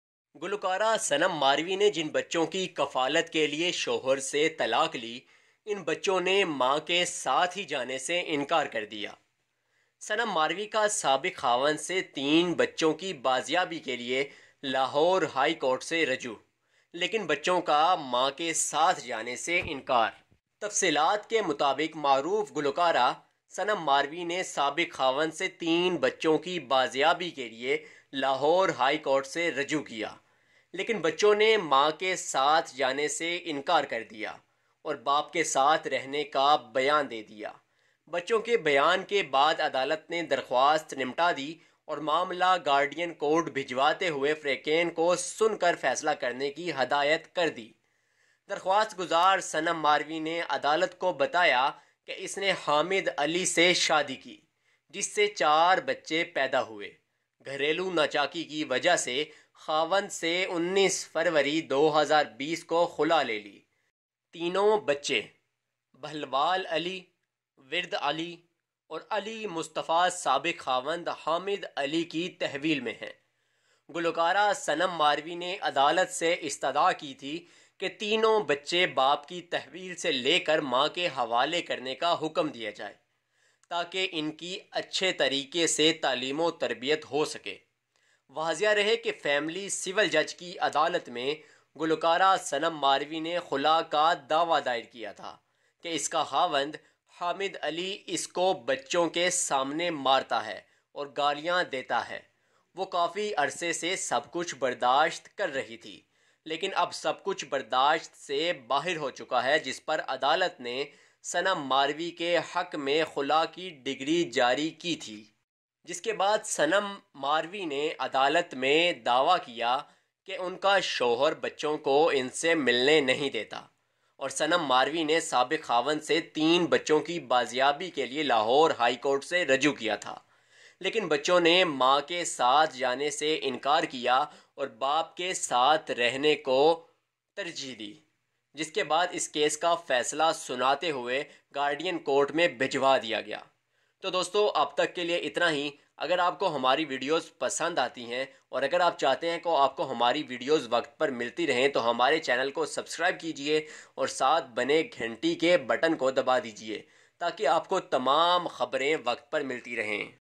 Singer Sanam Marvi's children refuse to go with mother
Singer Sanam Marvi divorced her husband to support her children, but they refused to go with her mother. Sanam Marvi appeals to Lahore High Court for recovery of three children from ex-husband but children refuse to go with mother